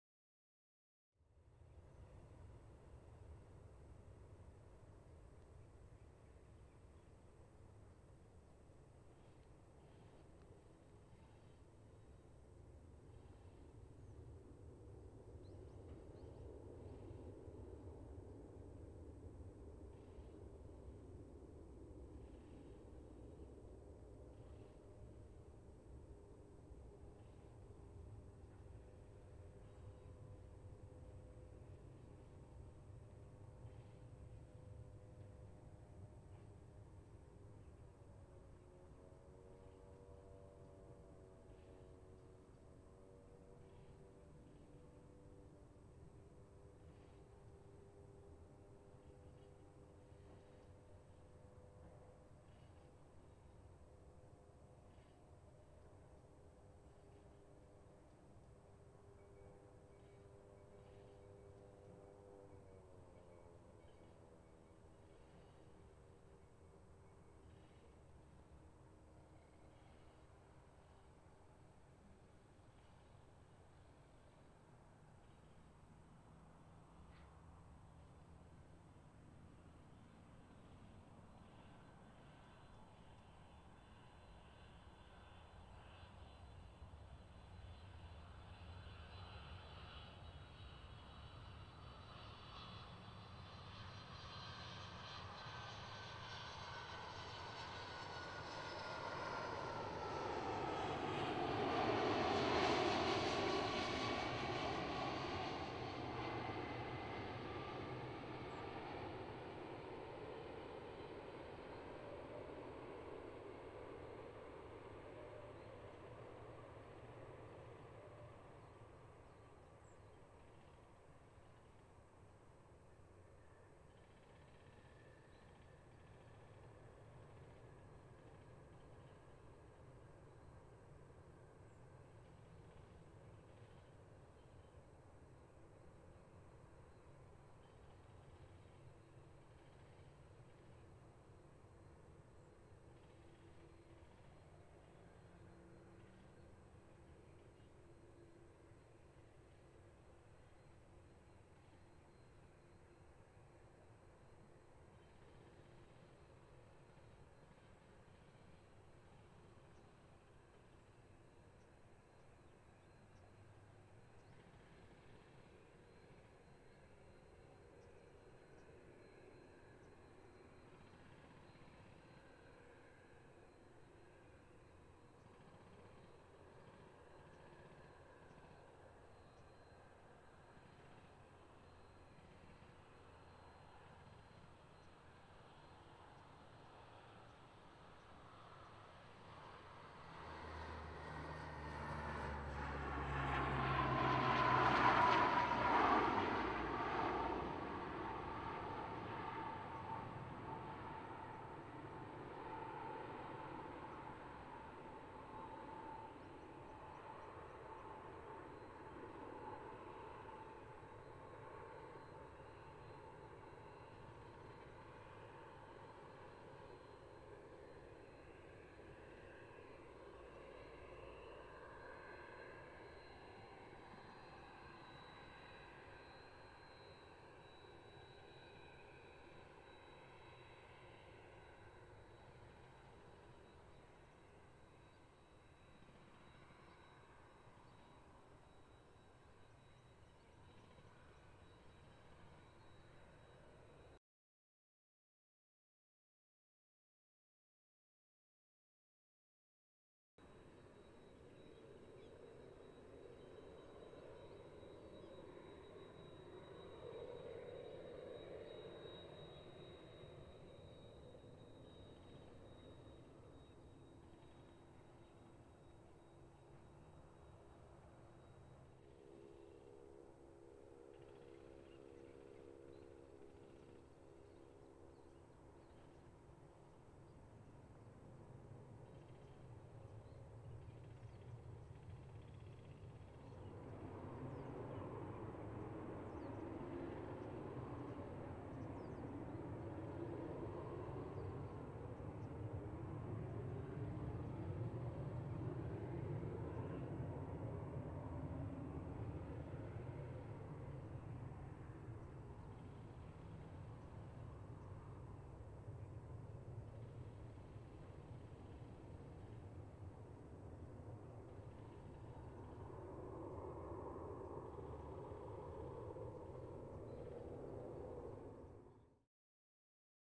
[那覇基地航空祭2015] T-4 芦屋レッドドルフィン帰投(那覇基地 2015年12月14日)
那覇基地航空祭2015の翌日に瀬長島でHC-X1000を使い撮った「T-4 芦屋レッドドルフィン帰投」の動画です。